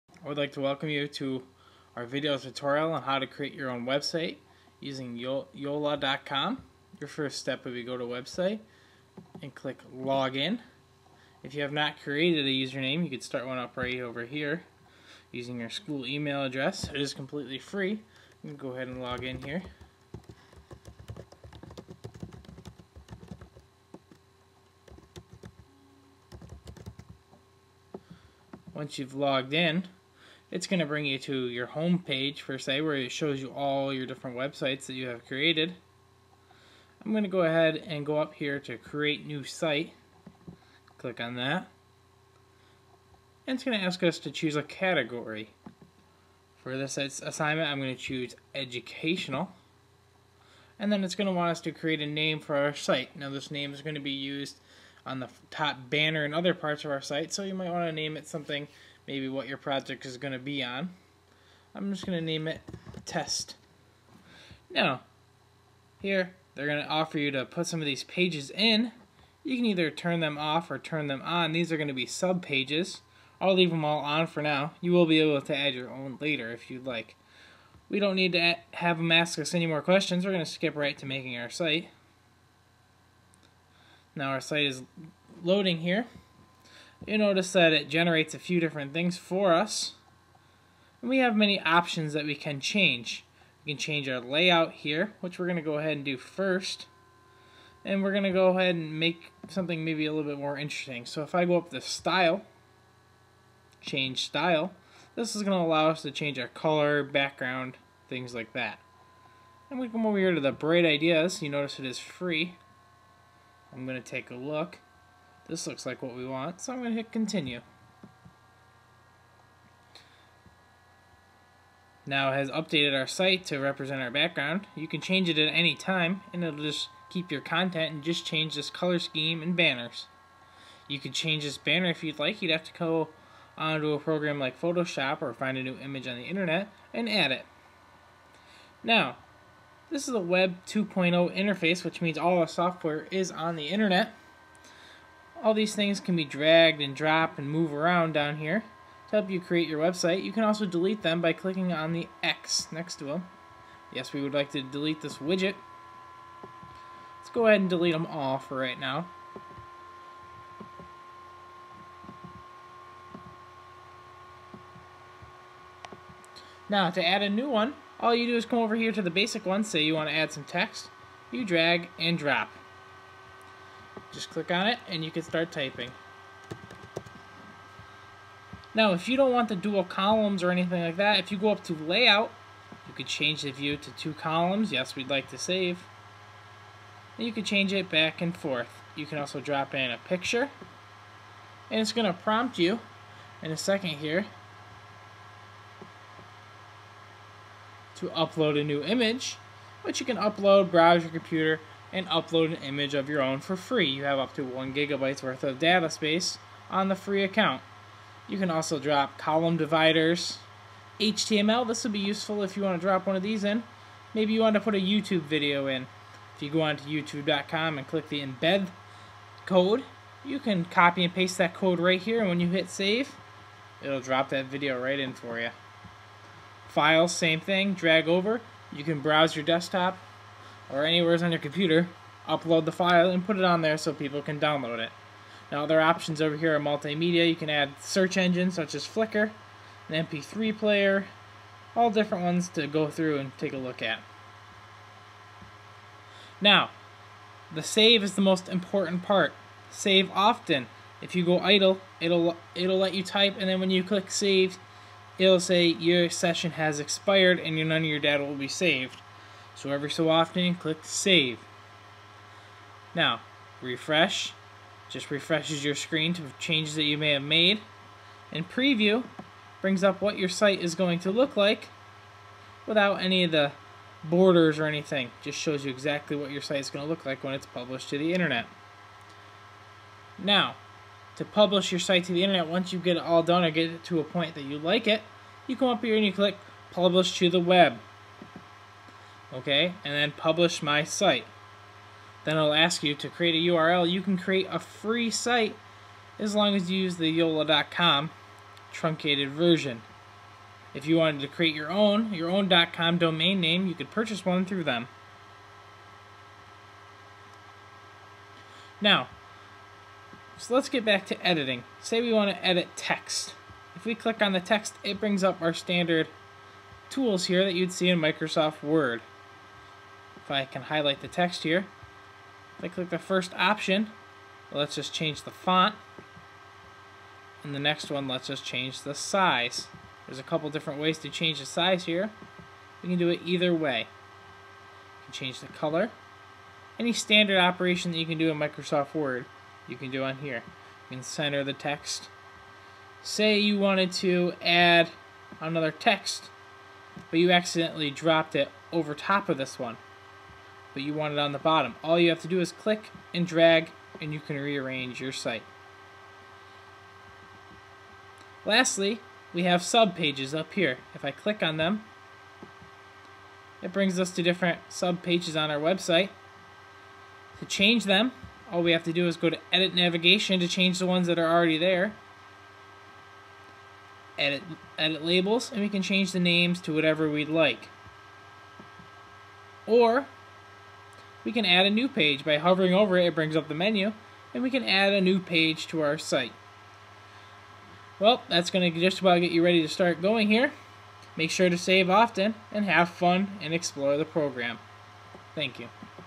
Yola Tutorial
A video tutorial of how to get started using Yola.com